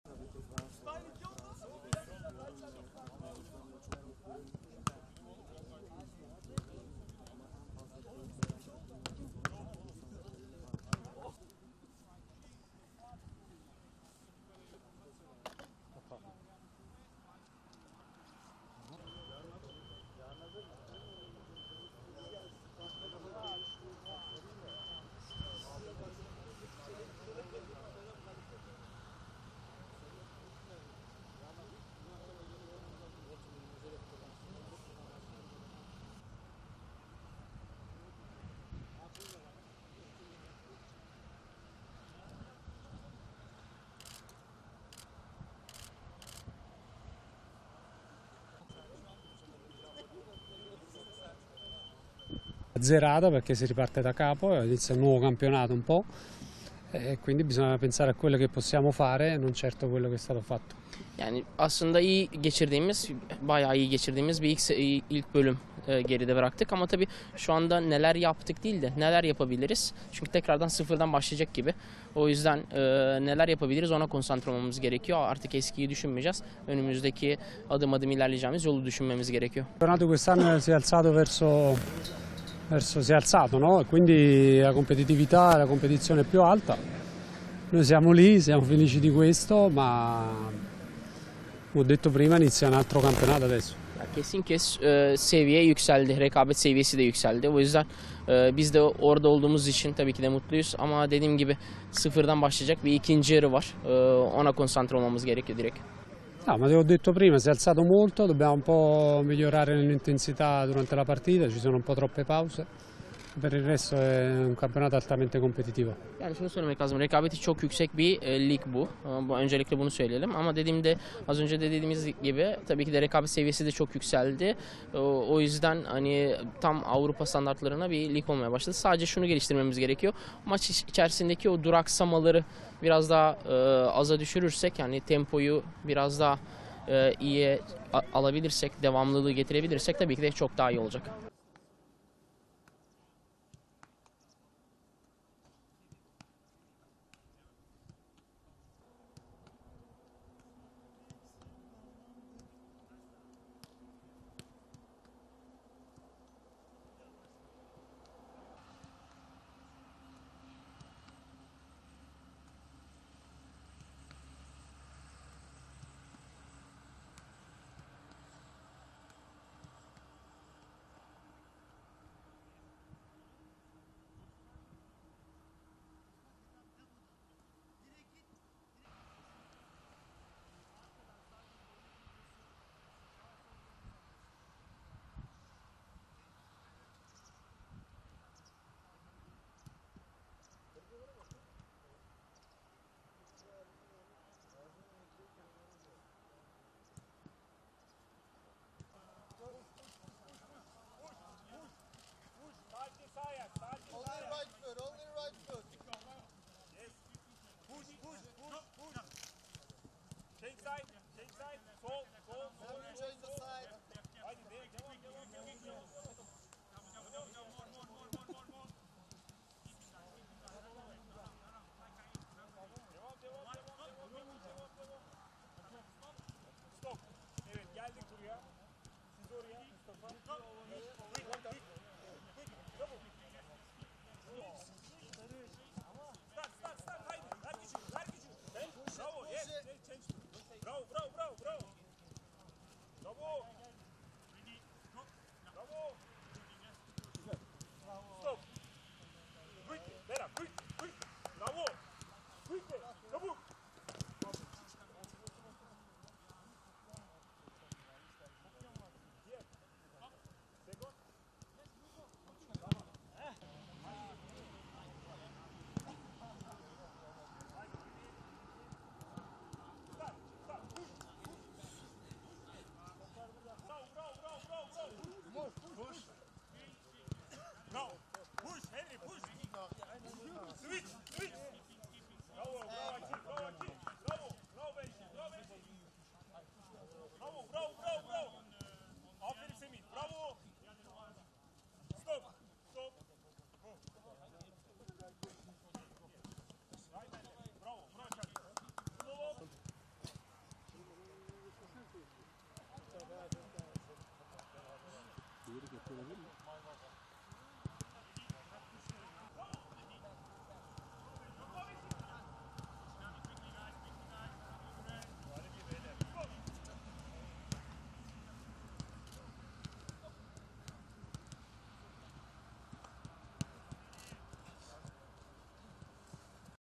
Vincenzo Montella: Süper Lig'de rekabet seviyesi yükseldi
“Neler yapabiliriz ona konsantre olmamız gerekiyor”

 Adana Demirspor Teknik Direktörü Vincenzo Montella, “Rekabeti çok yüksek bir lig. Öncelikle bunu söylemeliyim. Avrupa standartlarında bir lig olmaya başladı. Maç içindeki duraksamaları biraz daha aza düşürürsek, tempoyu biraz iyiye alıp, devamlılığı alabilirsek çok daha iyi olacak” dedi.

Dünya Kupası nedeniyle liglere verilen arada kamp çalışmalarına Antalya’nın Belek turizm merkezinde devam eden Adana Demirspor, hazırlıklarına akşam saatlerinde yaptığı antrenmanla devam etti. Koşu çalışmalarıyla başlayan antrenman, kuvvet çalışmalarıyla devam etti. Antrenmanın basına kapalı olarak yapılan bölümünde ise taktik çalışmaların gerçekleştirildiği öğrenildi. Lacivert-mavili ekipte teknik Direktör Vincenzo Montella, antrenman öncesinde basın mensuplarına açıklamalarda bulundu.

Kamp çalışmalarının oldukça iyi geçtiğini ifade eden Montella, “Aslında baya iyi geçirdiğimiz bir ilk dönemi geride bıraktık. Ancak şu anda ne yaptık değil, neler yapabiliriz ona bakıyoruz. Çünkü sıfırdan başlayacak gibi. O yüzden neler yapabiliriz ona konsantre olmamız gerekiyor. Eskiyi düşünmeyeceğiz. Önümüzde adım adım yürüyeceğimiz yola bakacağız” ifadelerini kullandı.

“SÜPER LİG’DE REKABET SEVİYESİ YÜKSELDİ”

Süper Lig’de seviyenin yükseldiğini ifade eden İtalyan teknik adam, “Süper Lig’de rekabet seviyesi yükseldi. O yüzden biz de orada olduğumuz için mutluyuz. Ama dediğim gibi sıfırdan başlayacak bir ikinci yarı var. Doğrudan ona konsantre olacağız. Rekabeti çok yüksek bir lig. Öncelikle bunu söylemeliyim. Avrupa standartlarında bir lig olmaya başladı. Maç içindeki duraksamaları biraz daha aza düşürürsek, tempoyu biraz iyiye alıp, devamlılığı alabilirsek çok daha iyi olacak” diye konuştu.